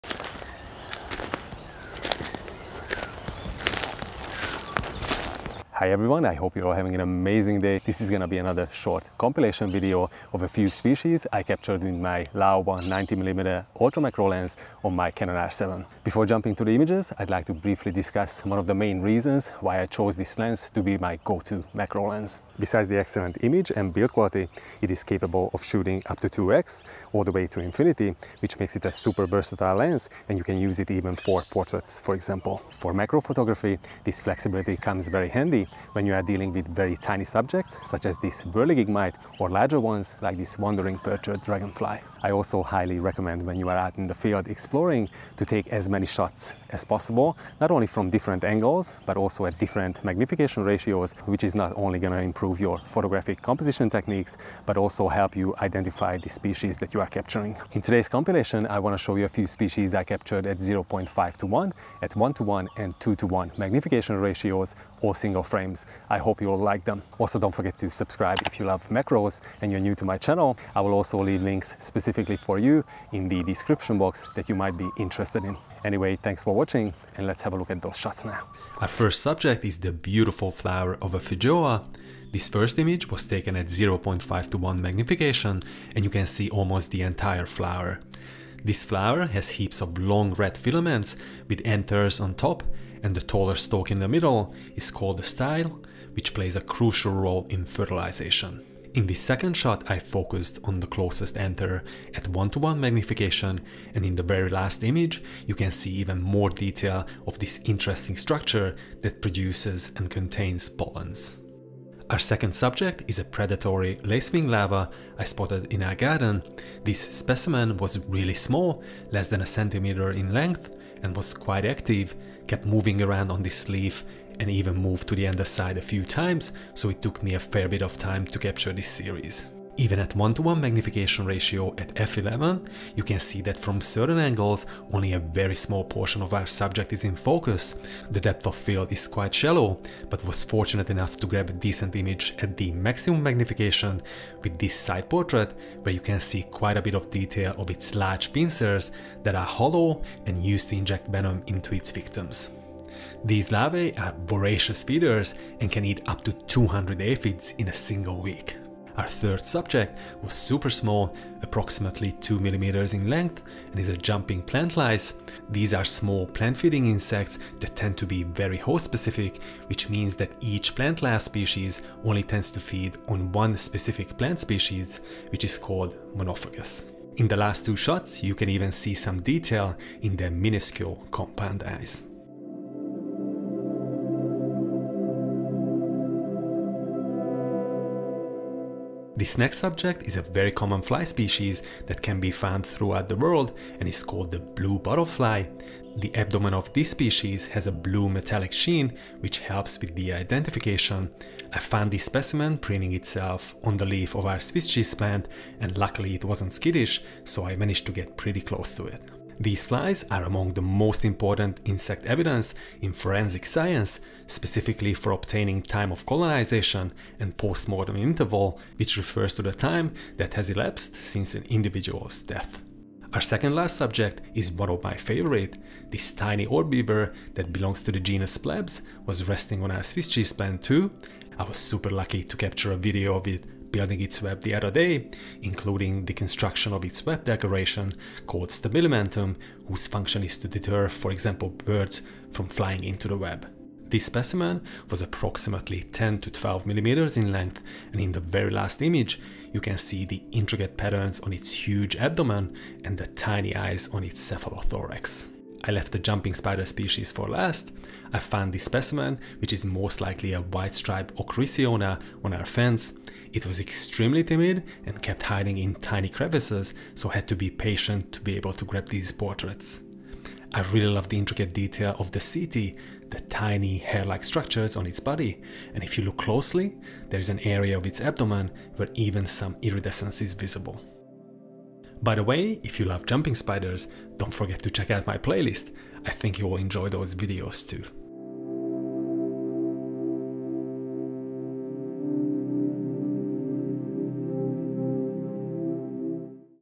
Why the Laowa 90mm is my go-to Macro Lens in the Field
Short macro compilation video showcasing the versatility of the Laowa 90mm Ultra Macro Lens.